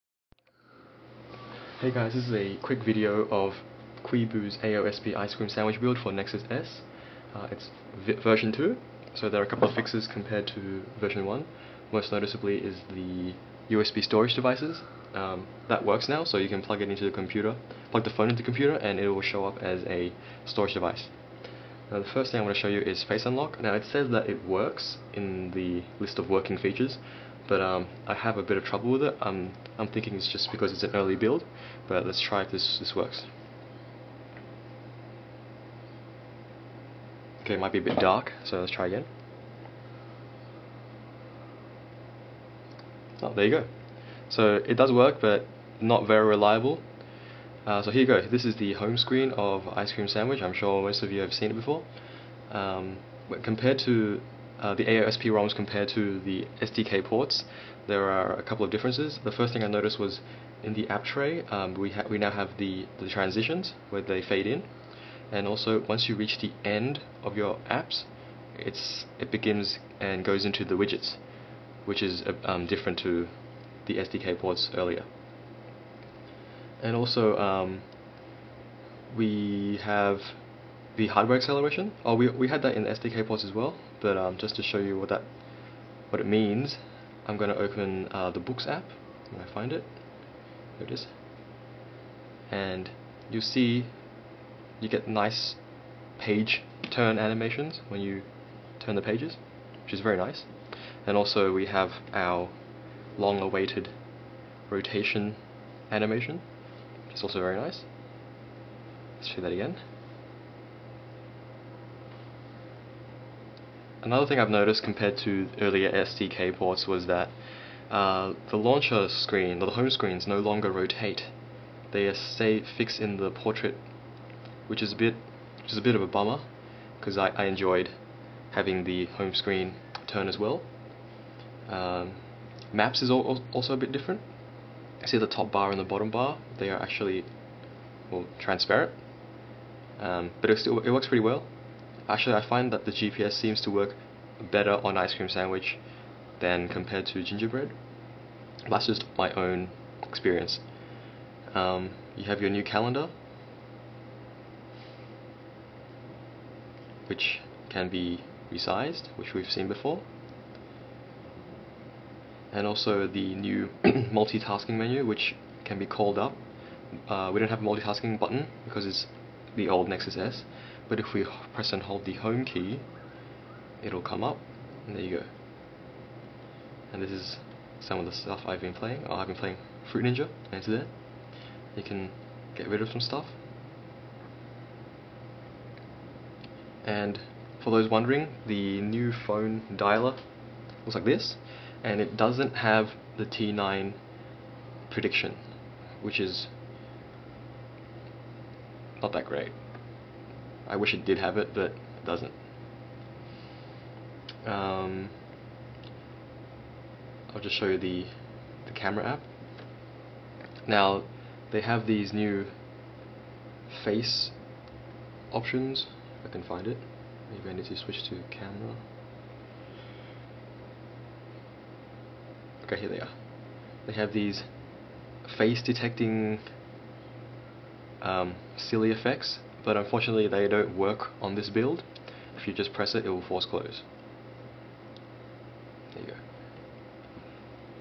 Kwiboo's AOSP Ice Cream Sandwich ROM v2 for Nexus S
A quick video demo of Kwiboo's AOSP Ice Cream Sandwich ROM v2. It's very stable for a WIP and can be used as your daily driver. Here's the link to the thread on xda